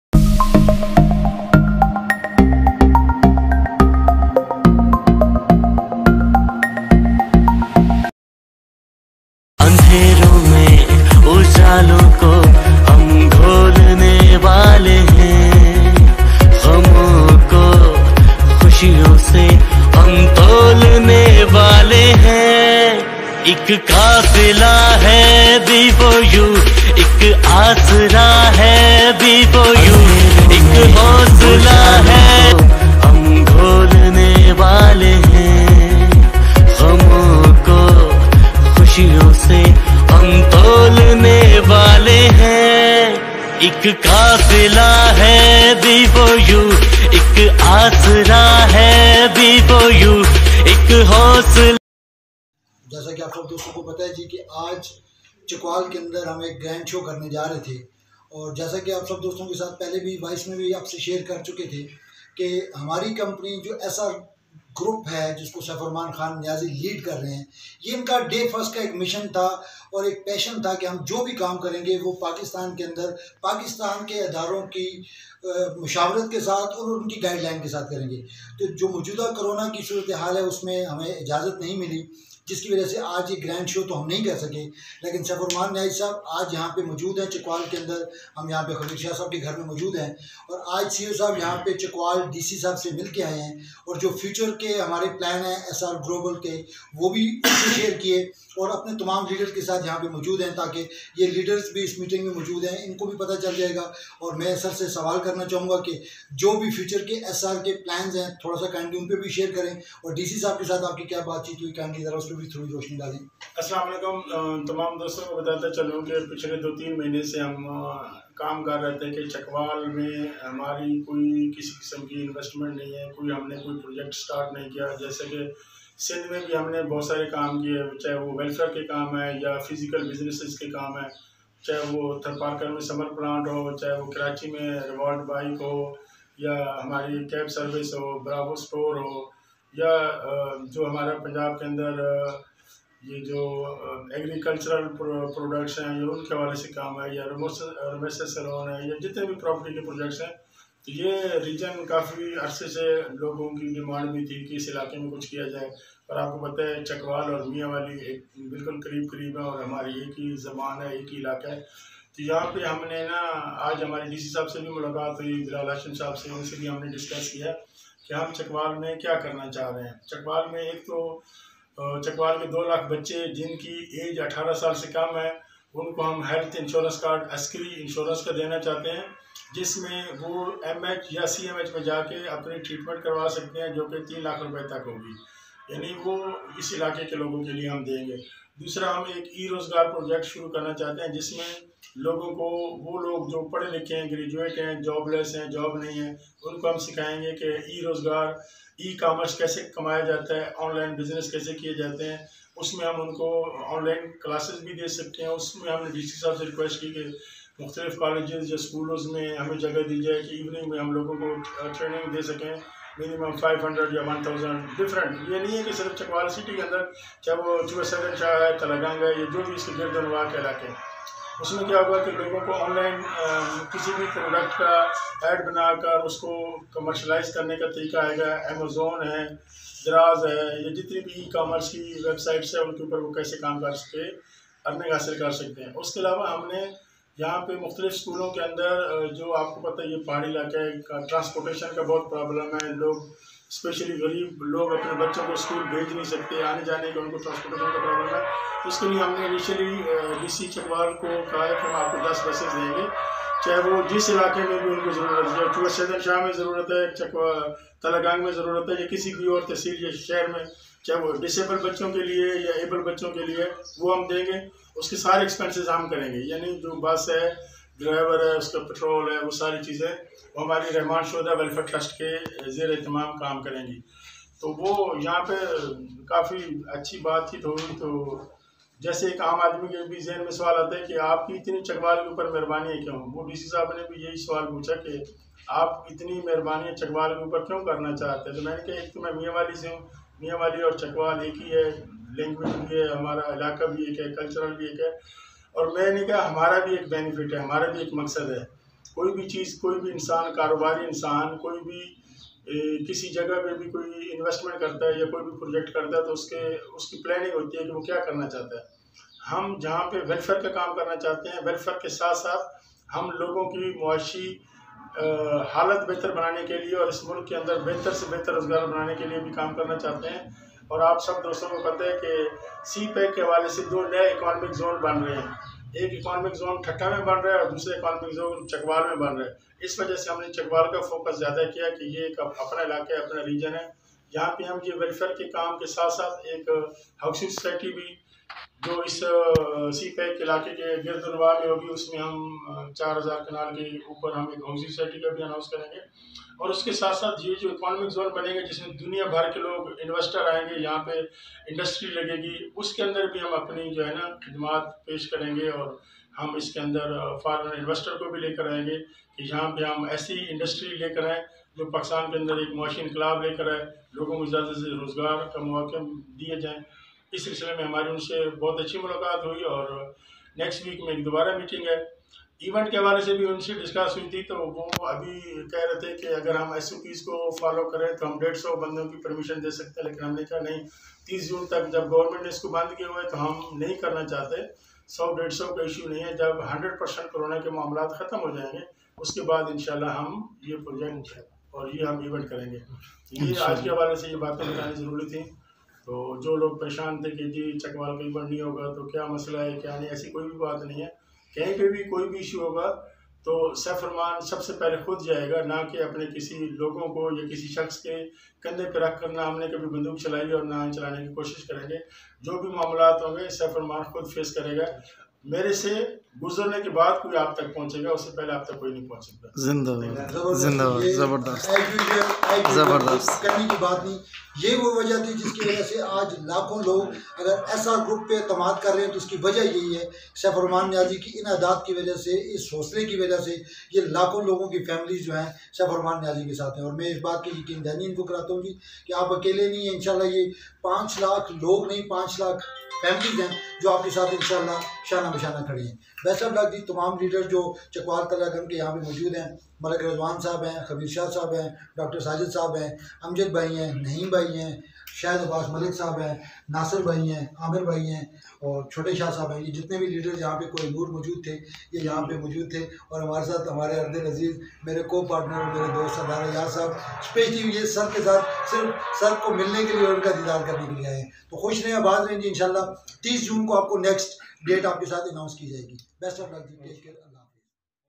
B4U Mega Event Chakwaal Cancelled || B4U GLOBAL SERIES
As salam o alekum friends you are watching ARSHAD HAMEED ONLINE why chakwaal event cancelled and big big news about chakwaal from saif ur rehman sab listen care fully so don't skip the video and like share and subscribe our channel for more updates .